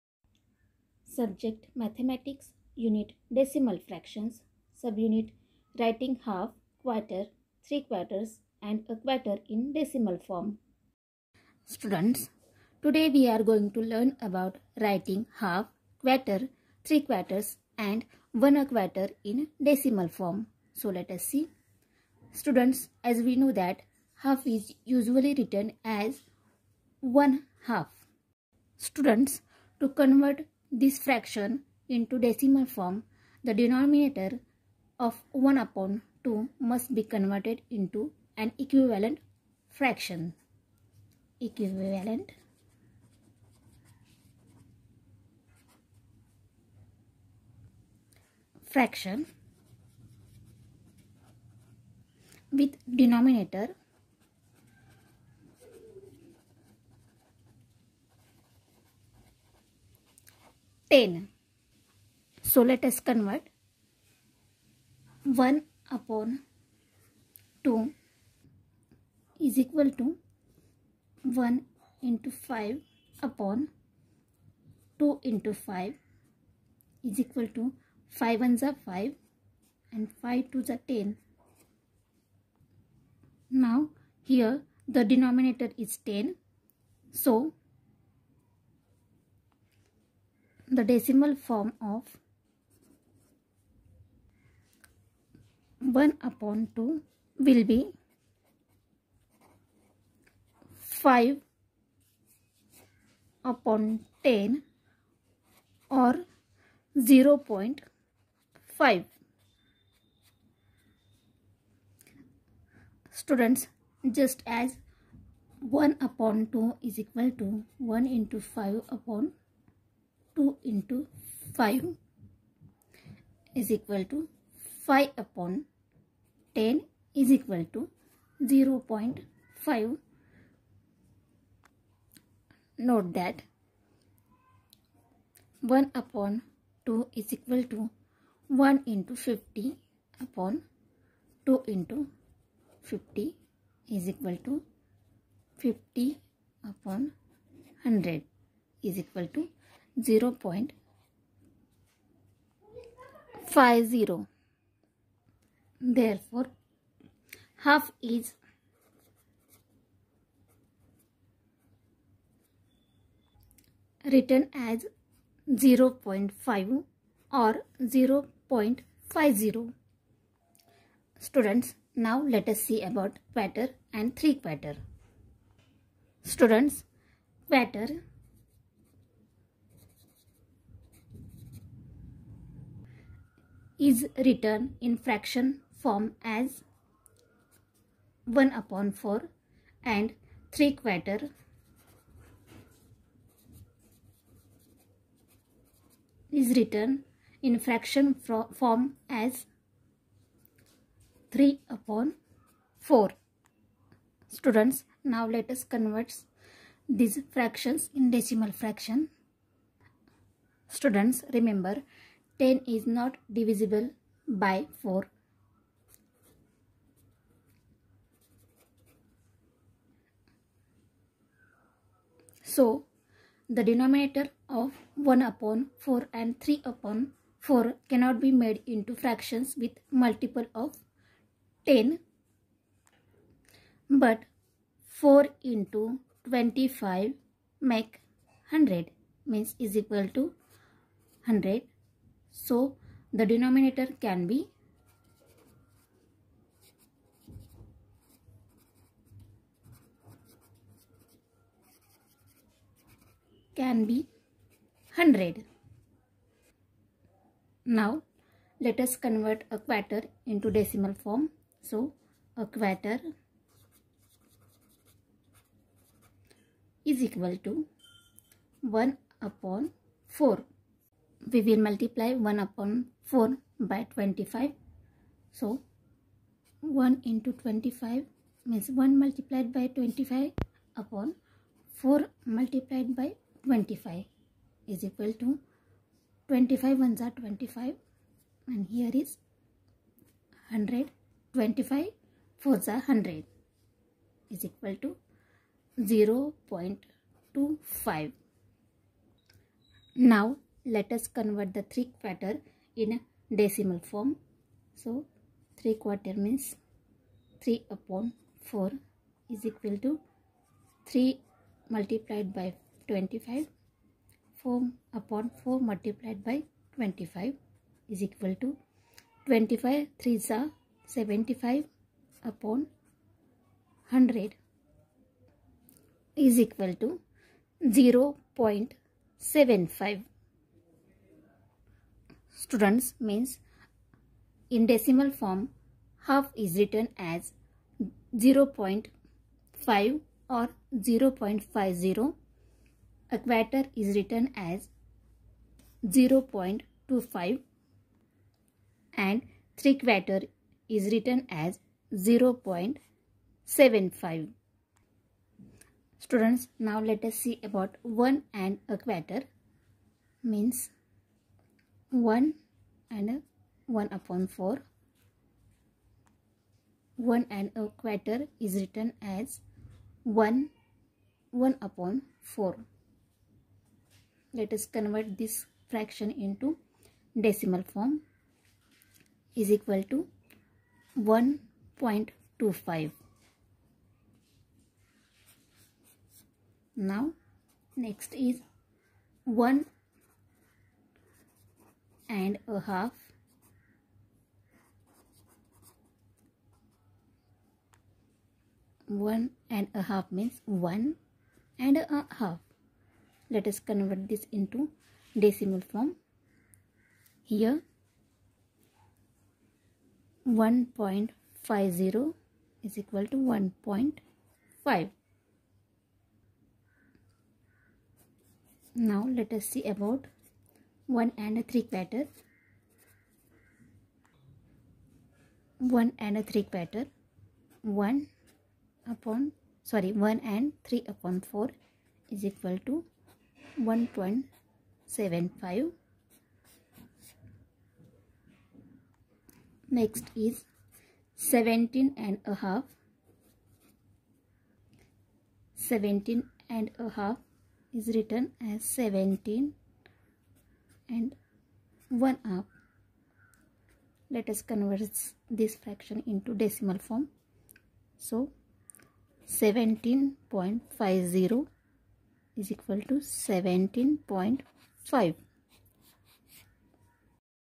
Decimal fractions- Writing half, quarter.....in decimal form - Std-5th
Writing quarter, half quarter, and three quarter in decimal form